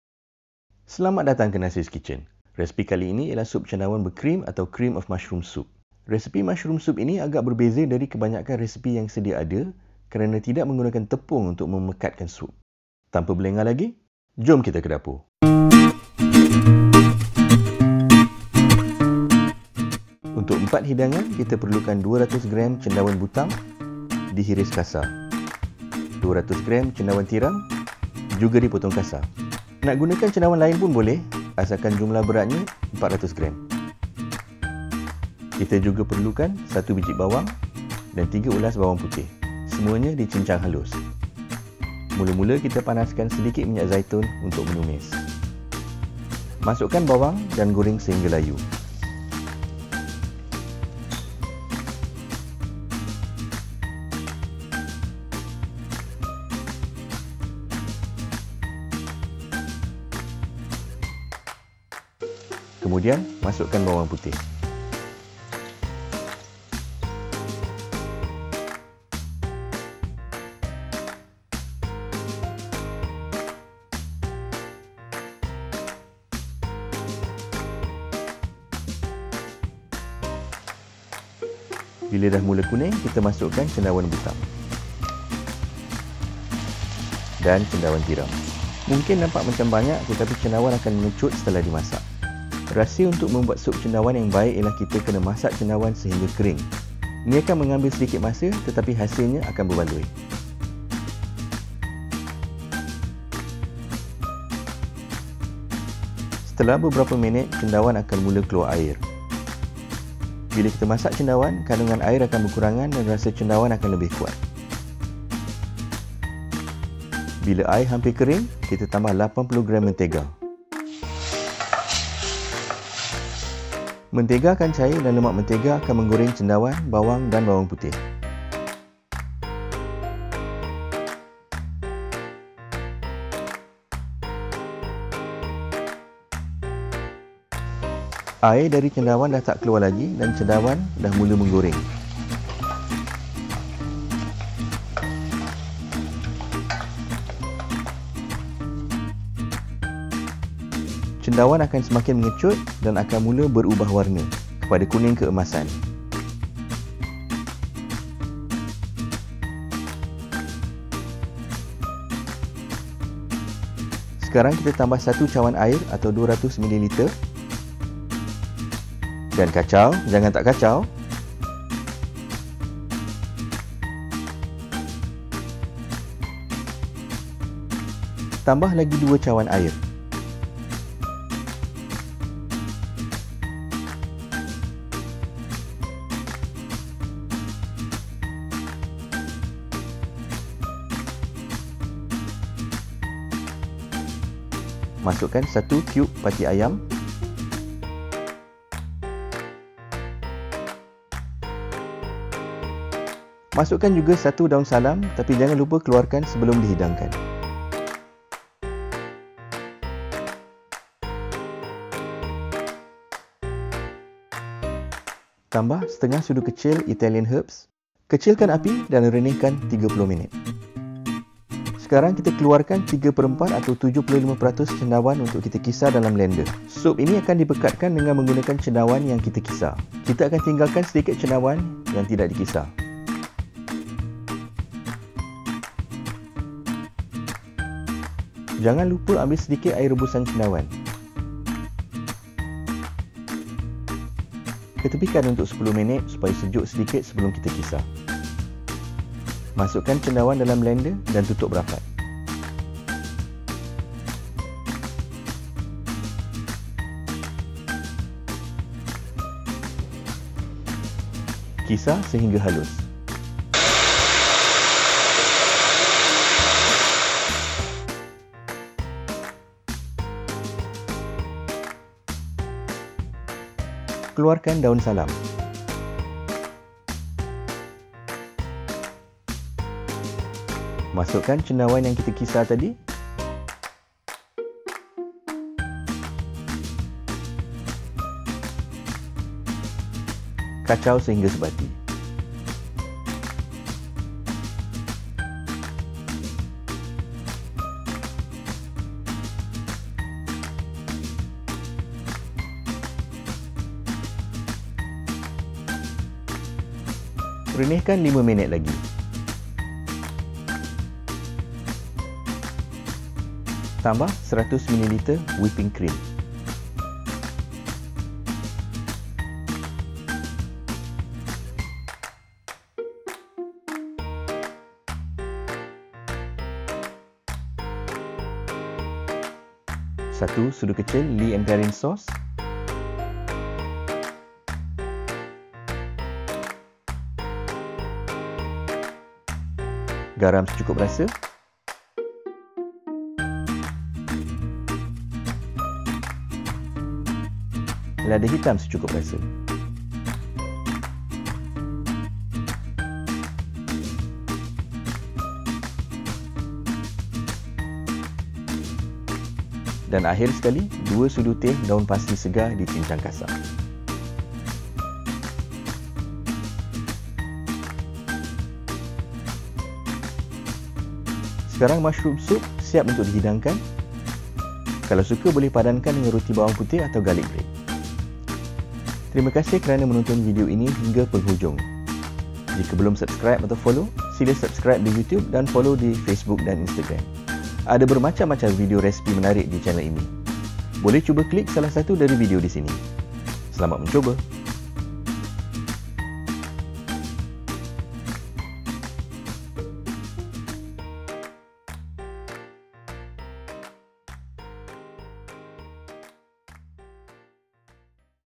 Resepi Sup Cendawan Berkrim Simple - Cream of Mushroom Soup
Resepi Sup Cendawan Berkrim Simple in tidak menggunakan tepung untuk memekatkan sup tetapi cendawan akan dikisar dengan blender sehingga halus.

Rahsia resepi sup cendawan berkrim ini ialah memastikan cendawan masak betul-betul sehingga semua airnya keluar.

Sup Cendawan berkrim  sesuai untuk dipadankan dengan roti bakar bawang putih atau garlic bread.

BAHAN-BAHAN
200g cendawan tiram
200g cendawan butang
1 biji bawang besar
3 ulas bawang putih
Minyak zaitun untuk menumis
80g mentega
1 kiub pati ayam
3 cawan (750ml) air
100ml whipping cream
1/2 sudu teh Italian herbs
2 sudu teh parsley
1 daun salam
Garam
Lada Hitam

TEKS PENUH VIDEO
Selamat Datang ke Nasser's Kitchen. Resepi kali ini ialah Sup Cendawan Berkrim atau Cream of Mushroom soup. Resepi mushroom soup ini agak berbeza dari kebanyakan resepi yang sedia ada kerana tidak menggunakan tepung untuk memekatkan sup. Tanpa berlengah lagi, Jom kita ke dapur!

Untuk 4 hidangan kita perlukan 200g cendawan butang, di hiris kasar

200g cendawan tiram, juga dipotong kasar

Nak gunakan cendawan lain pun boleh, asalkan jumlah berat cendawan 400g

Kita juga perlukan 1 biji bawang dan 3 ulas bawang putih, semua dicincang halus.

Mula-mula kita panaskan sedikit minyak zaitun untuk menumis.

Masukkan bawang dan goreng sehingga layu

Kemudian masukkan bawang putih

Goreng sehingga mula berwarna kuning, pastikan api tidak terlalu kuat supaya bahan tumisan tidak hangus.

Bila dah kuning kita masukkan cendawan butang

dan cendawan tiram

Mungkin nampak macam banyak tetapi cendawan akan mengecut setelah dimasak.

Rahsia untuk membuat sup cendawan yang baik ialah kita kena masak cendawan sehingga kering, ini akan mengambil sedikit masa tapi hasilnya akan berbaloi.

Setelah beberapa minit cendawan akan mula keluar air. Bila kita masak cendawan , kandungan air akan berkurangan dan rasa cendawan akan lebih kuat.

Bila air hampir nak kering, kita tambah 80g mentega

Mentega akan cair dan lemak mentega akan menggoreng cendawan, bawang dan bawang putih.

Air dari cendawan dah tak keluar lagi dan cendawan dah mula digoreng.

Langkah ini penting, kita kena pastikan cendawan betul-betul masak.

Cendawan akan semakin mengecut dan akan mula berubah warna, kepada kuning keemasan.

Sekarang kita tambah 1 cawan air dan kacau, jangan tak kacau.

Tambah lagi 2 cawan air.

Masukkan 1 kiub pati ayam.

Masukkan juga 1 daun salam tapi jangan lupa keluarkan sebelum di hidangkan.

Tambah 1/2 sudu kecil Italian Herbs, kecilkan api dan renihkan 30 minit.

Sekarang kita keluarkan 3/4 atau 75% cendawan untuk kita kisar dalam blender, sup ini akan dipekatkan dengan menggunakan cendawan yang kira kisar. Kita akan tinggalkan sedikit cendawan yang tidak dikisar.

Jangan lupa ambil sedikit air rebusan cendawan. Ketepikan untuk 10 minit supaya sejuk sedikit sebelum kita kisar.

Masukkan cendawan kedalam blender dan tutup rapat.

Kisar sehingga halus.

keluarkan daum salam.

Masukkan cendawan yang dikisar tadi. Kacau sehingga sebati.

Renihkan 5 minit lagi

Tambah 100ml whipping cream.

1 sudu kecil Lea & Perrin's sauce

lada hitam secukup rasa

dan akhir sekali 2 sudu teg daun parsley segar dicincing kasar.

Sekarang mushroom soup siap untuk dihidangkan. Kalau suka boleh padankan dengan roti bawang putih atau garlic bread.

Terima kasih kerana menonton video ini hingga ke penghujung. Ada bermacam-macam video resepi menarik dichannel ini,
boleh cuba klik salah satu dari video disini. Selamat Mencuba!